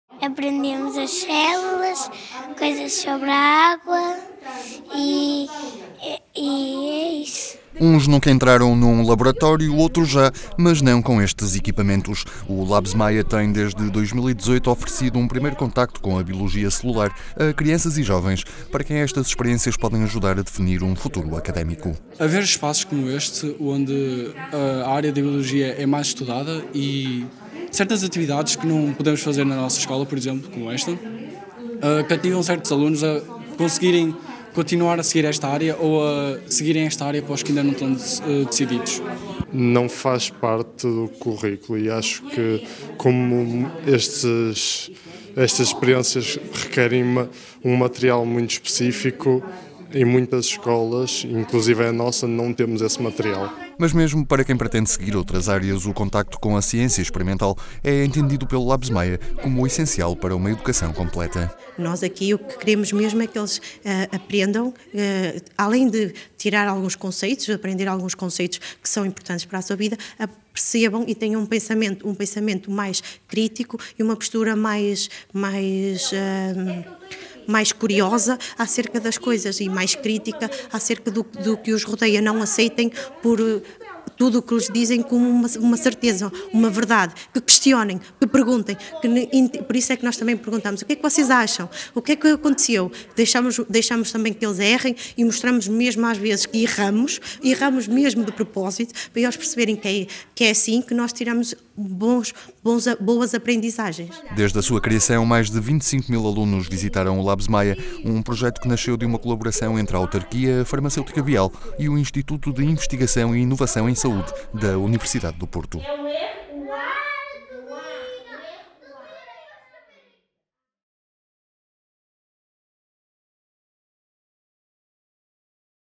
Entre lâminas, lamelas, pipetas e microscópios “fazem-se cientistas”
Uns nunca entraram num laboratório, outros já, mas “não num assim” e entre lâminas, lamelas, pipetas e microscópios descobrem células, calculam hipóteses de diagnosticar cancros e “fazem-se cientistas” no LABS.Maia, um projeto para “ajudar a pensar fora da caixa”.